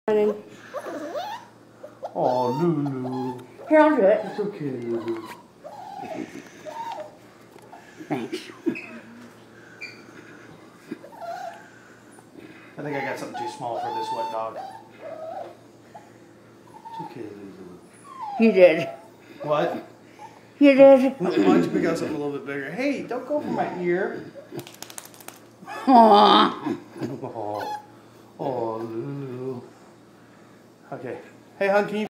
First Bath
Lulu the Miniature Schnauzer gets her very first bath.  She is so cute!